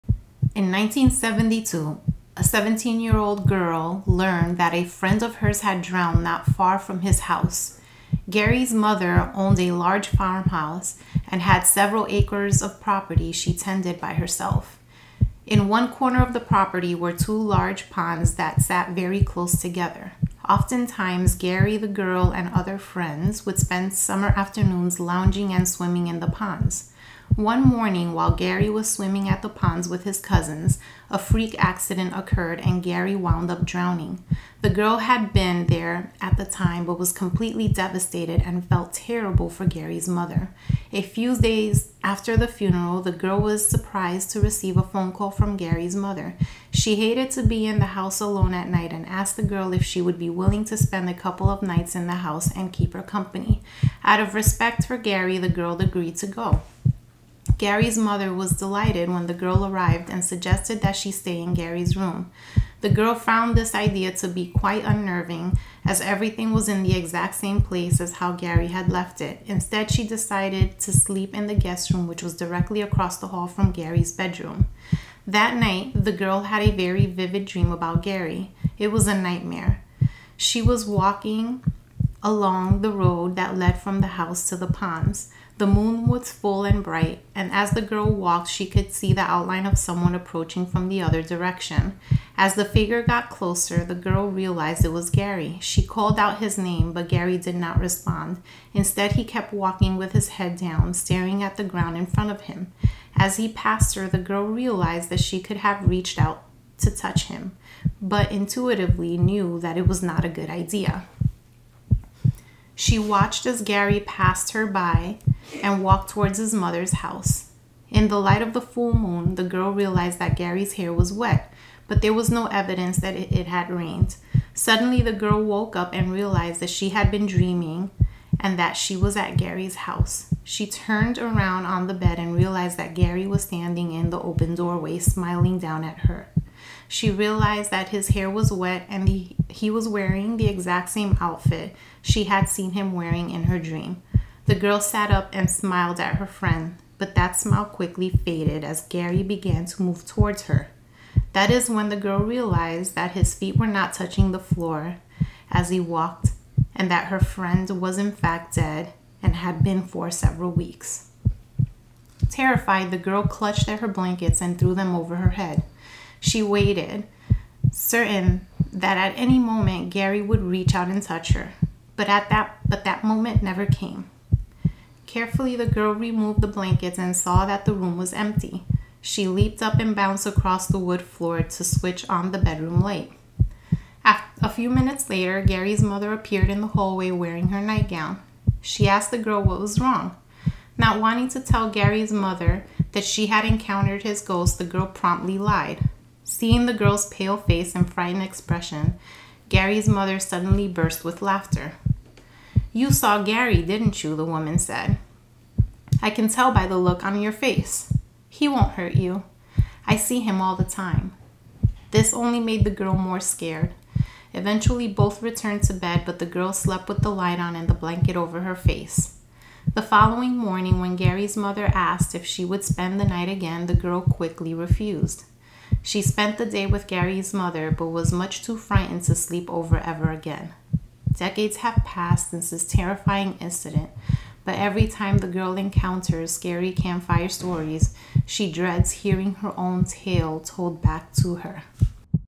Ghost Friend
This story combines the calming sound of a heartbeat with a terrifying story.